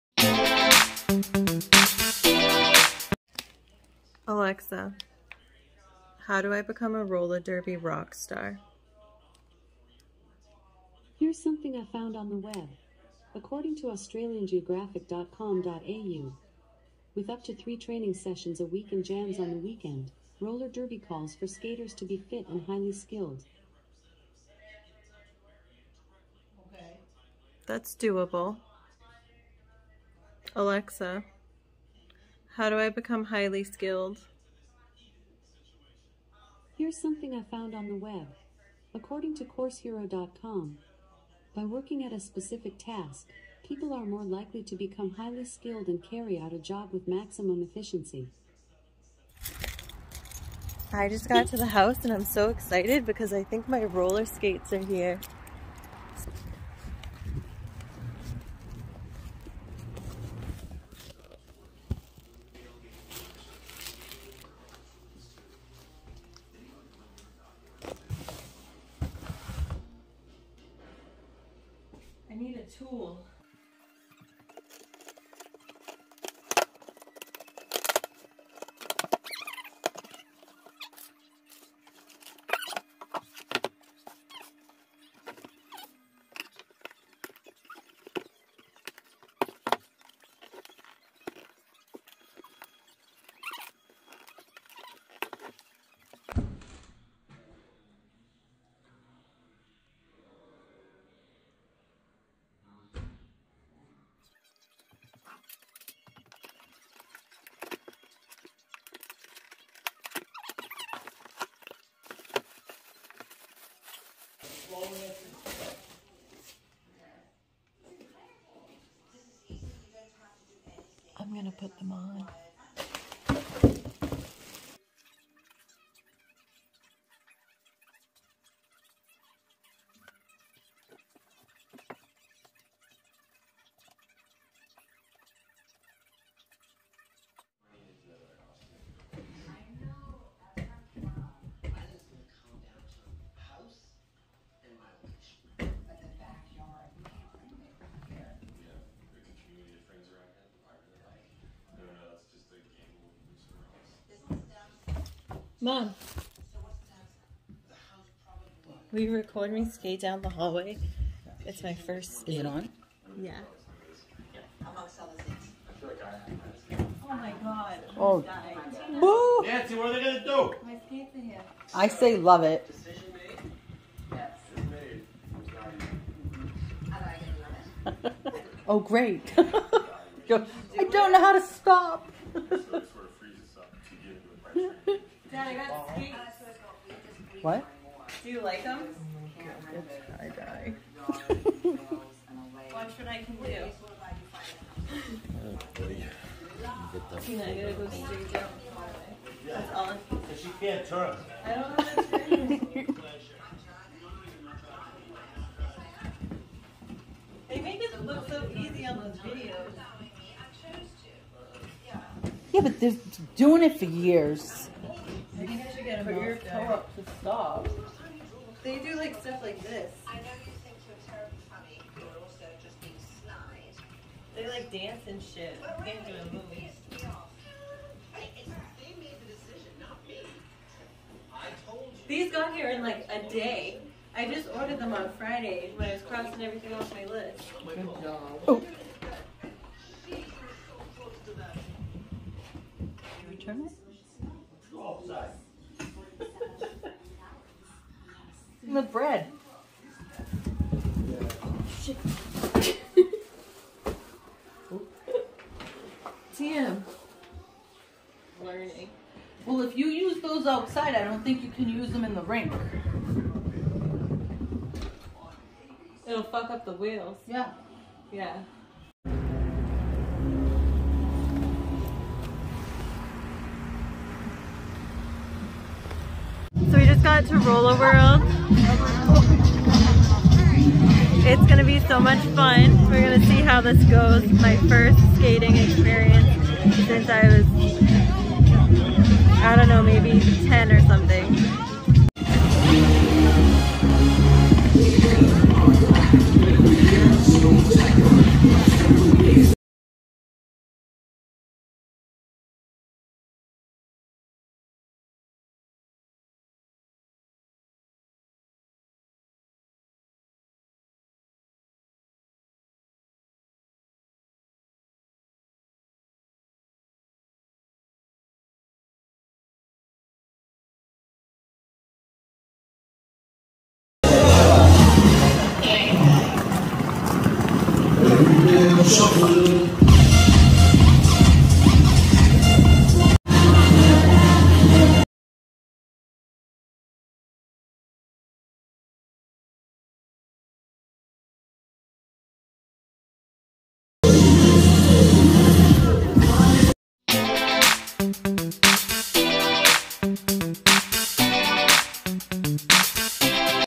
Roller Derby Rockstar
How do you become a Roller Derby Rockstar?

Step 1 : Learn how to skate

My skates have arrived and now it's practice time!
I really need to learn how to turn...I only fell one time so far
You can find me at Roller World on Tuesdays giving it my best shot 🙏

Also I learned how to extract and attach audio in the kinemaster app😅